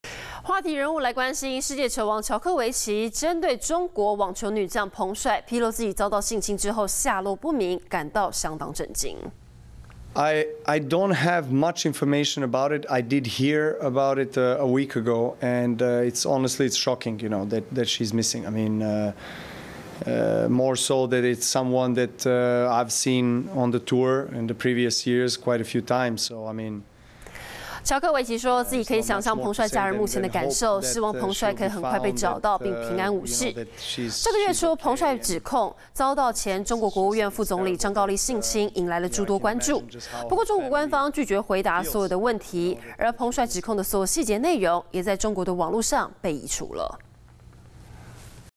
彭帥控遭高官性侵後失蹤 喬科維奇震驚｜華視新聞 20211116
「世界球王」喬科維奇，針對中國網球女將彭帥，披露自己遭性侵後下落不明一事表達震驚。喬科維奇說，自己可以想像彭帥家人目前的感受，很希望彭帥能快點被找到，並且平安無事。